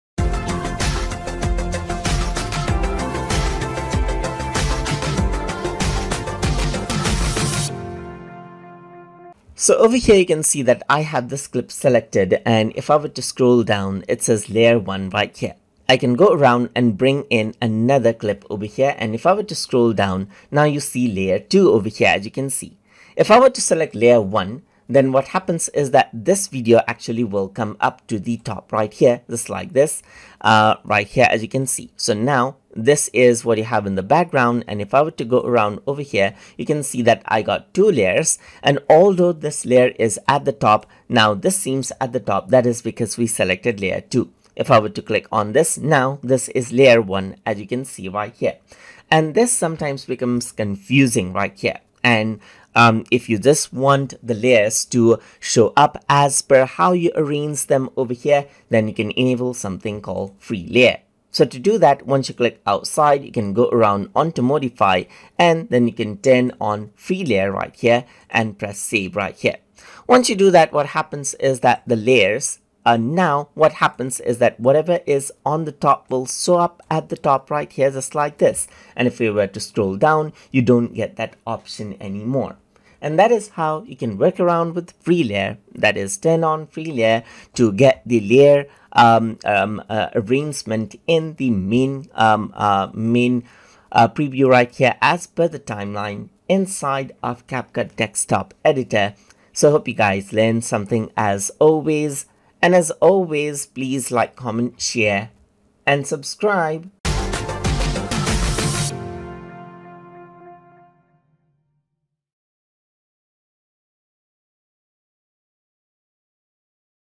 CapCut Desktop Editor - Lesson 85 - Free Layer
In this tutorial, we will be discussing about Free Layer in CapCut

Learn the basics of using CapCut including how to cut, edit, add effects and export videos.

This lesson has been made using CapCut in Google Chrome Web Browser but is applicable for desktop and mobile version of the application as well,